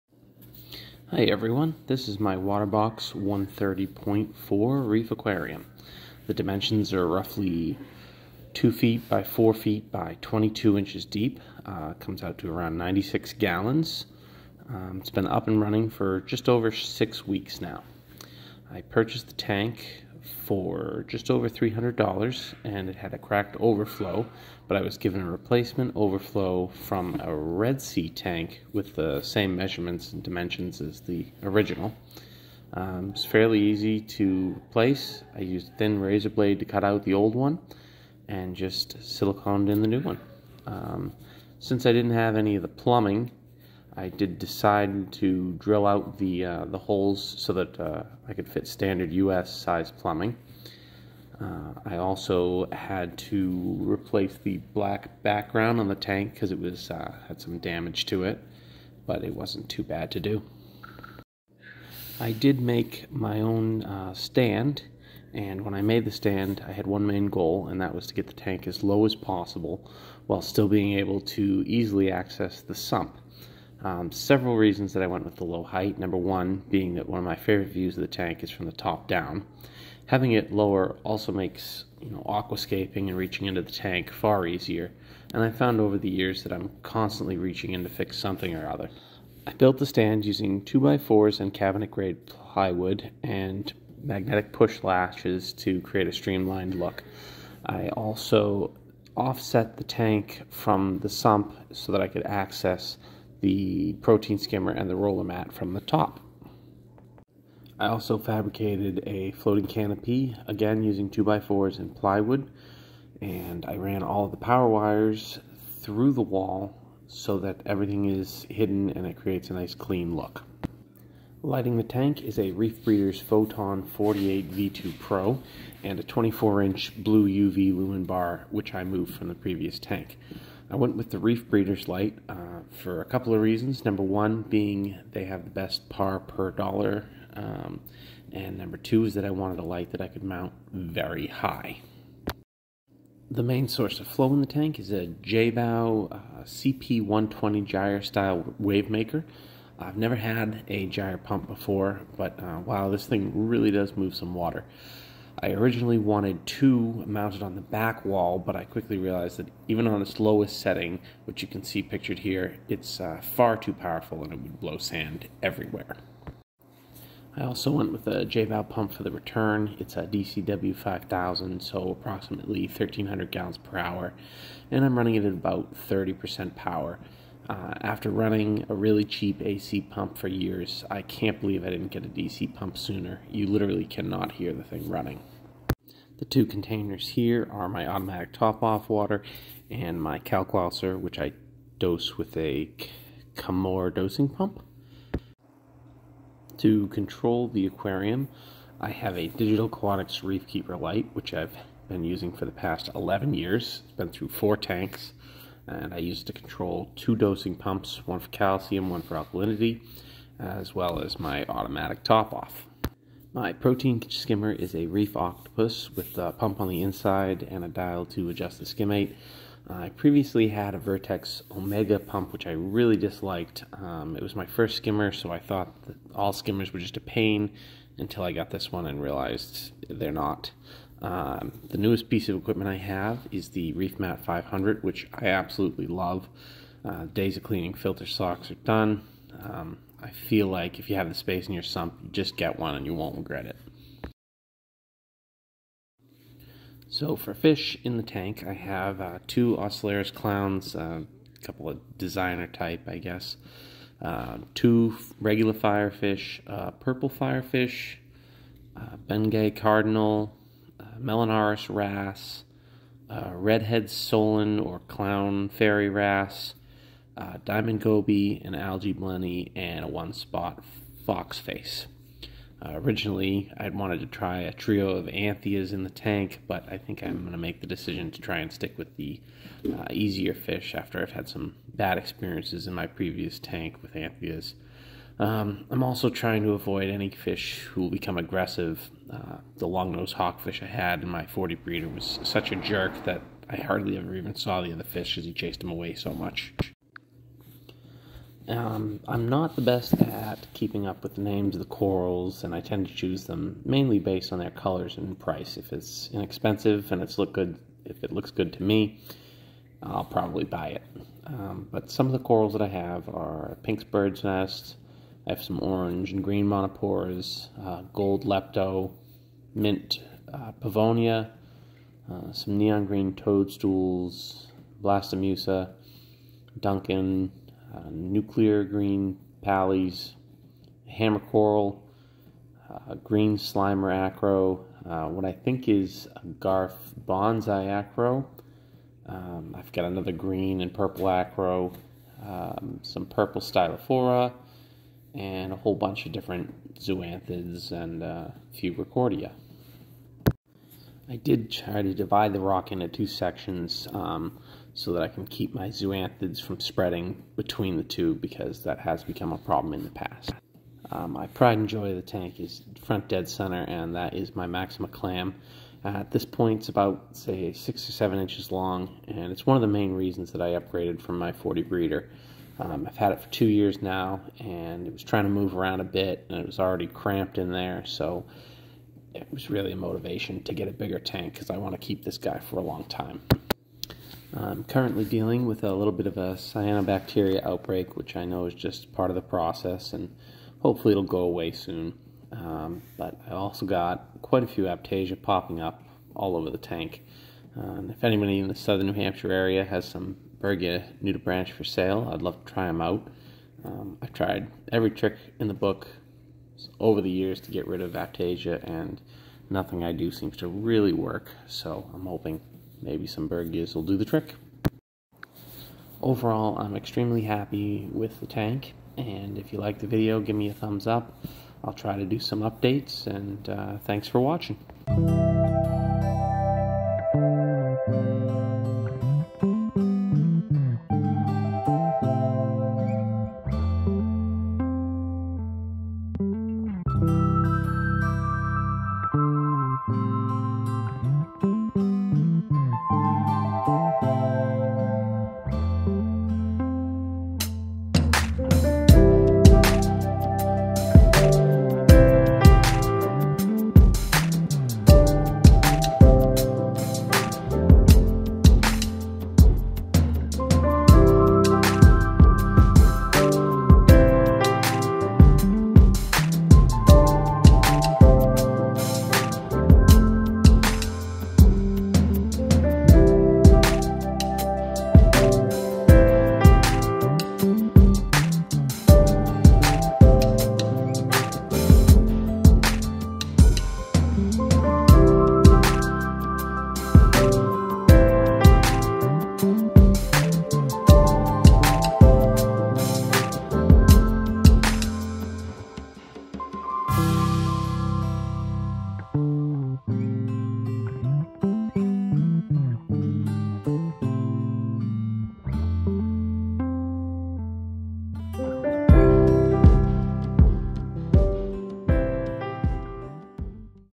New Waterbox 130.4 saltwater reef aquarium tank.
I have put together a complete overview of my new saltwater coral reef tank.

waterbox 130.4
96 Gallon
24" x 48" x 22"
Reefbreeders photon 48 v2 pro led
Reefmat 500 roller mat
reef octopus skimmer
jebao cp-120 gyre
jebao dcw-5000 return
Bulk reef supply titanium Heaters

Corals list:

Pink birdsnest
Orange and green montiporas
Gold lepto
Mint pavonia
Neon green toadstool
Blastomussa
Duncan
Nuclear green plays
Hammer coral
Green slimer
What I think is a garf bonsai acro
Another unknown acro
Purple stylophora
And assorted zoanthida and ricordea

Fish list:

2 ocellaris clowns
2 regular firefish
1 purple firefish
bengaii cardinalfish
Melanarus wrasse
Red head solon "clown" fairy wrasse
Diamond goby
Algae blenny
And a one spot foxface